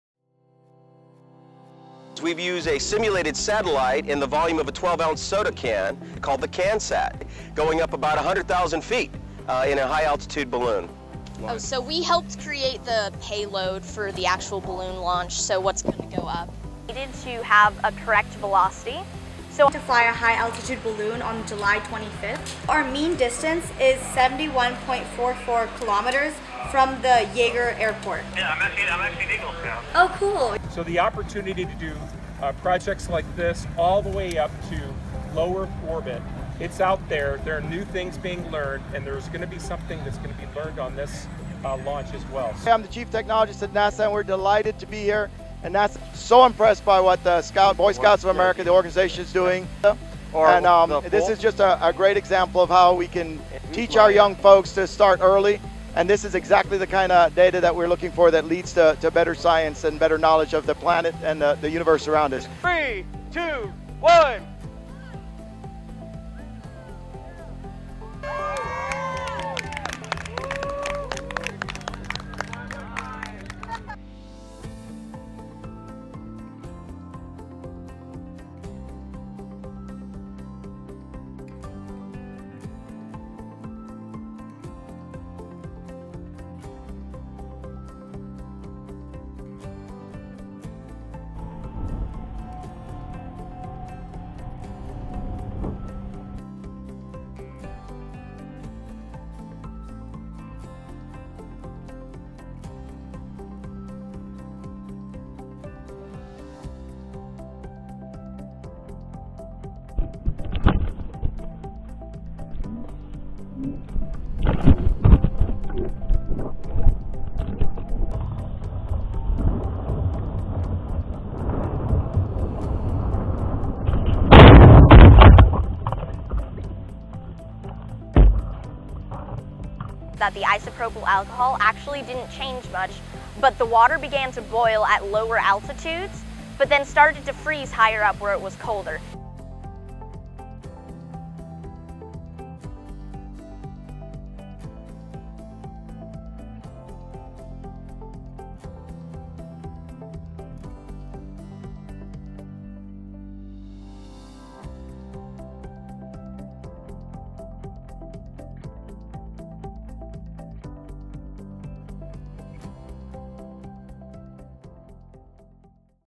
CanSat High Altitude Balloon launch at the Boy Scouts National Jamboree
Compilation of our CanSat High Altitude Balloon Project with the Venture Crew, Boy Scouts of America. Douglas Terrier, Chief Technologist of NASA and Greg Johnson, President of CASIS came out to speak in support of the hands-on learning in STEM education. Learning how to measure the world around them close to 100,000 ft.